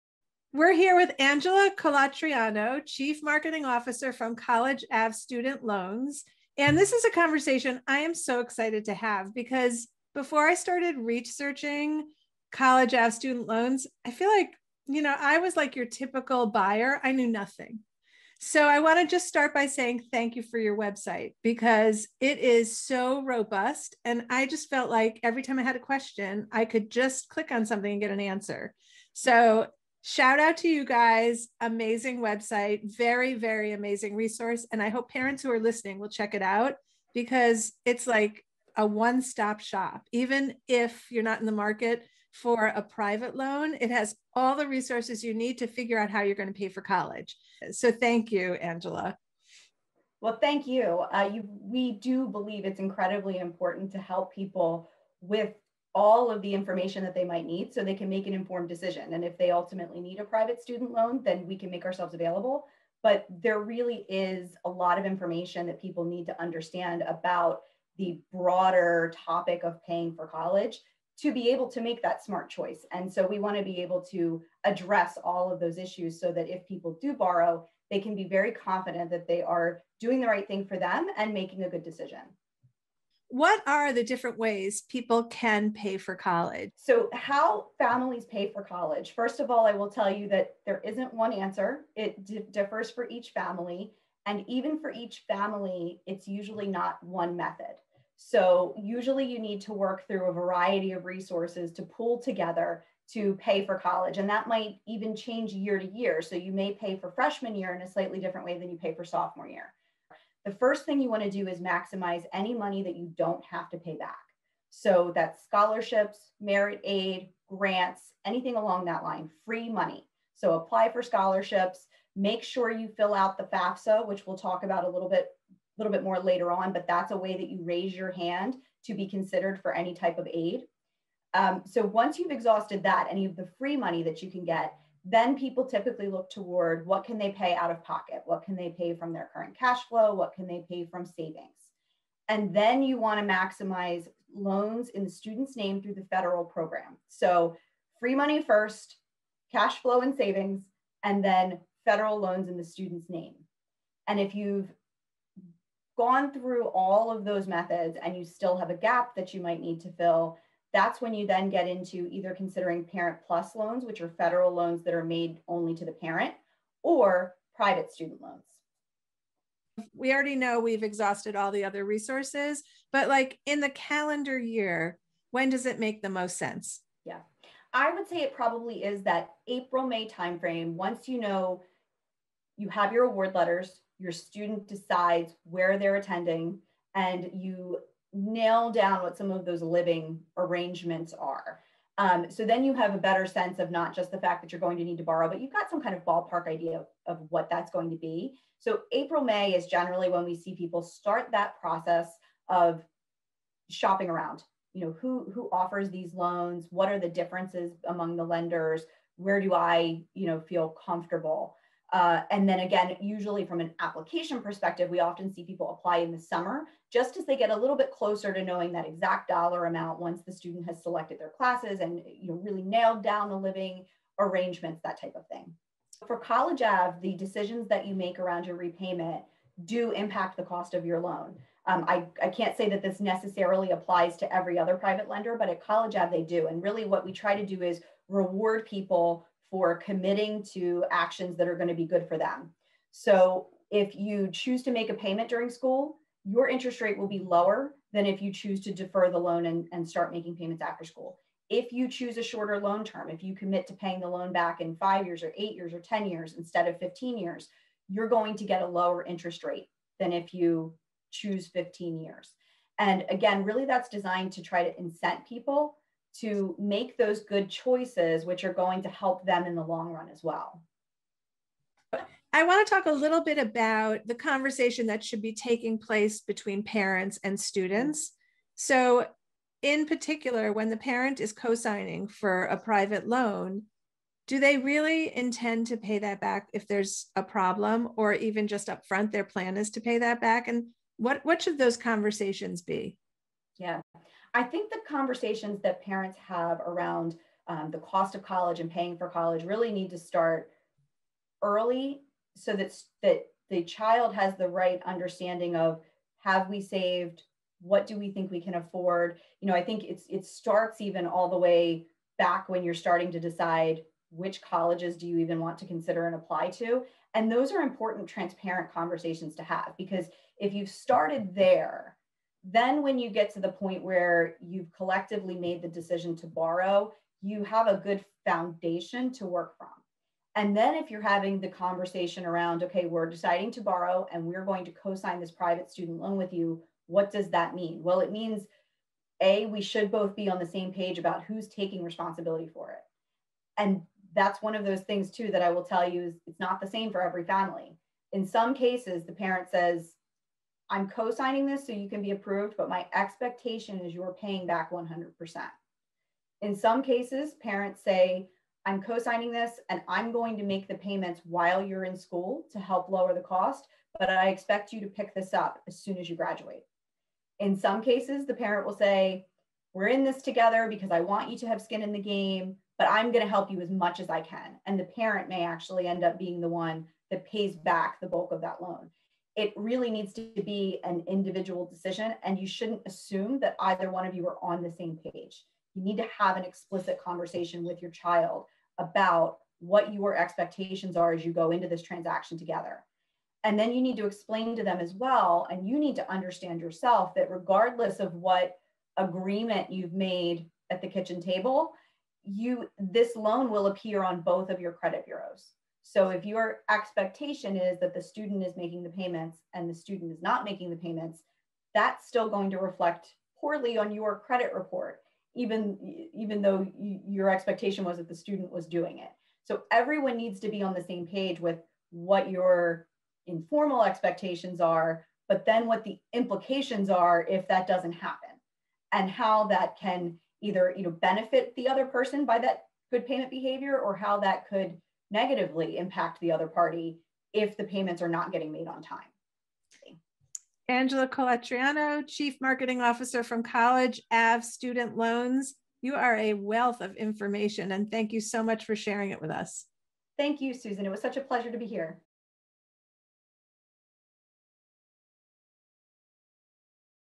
College Ave: Find the Best to Pay for College
If you’re looking for a robust resource about paying for college, look no further. College Ave offers:

·      Information about how to calculate the cost of college

·      The tools to calculate the best strategy to pay for college

·      A simple process for getting a loan

Every minute you spend on the College Ave website is a good investment!

According to advisors from College Ave, the best way to pay for college is to use money that you don’t have to pay back. Here’s your chance to win a $1000 scholarship to do just that! Click here to enter that drawing.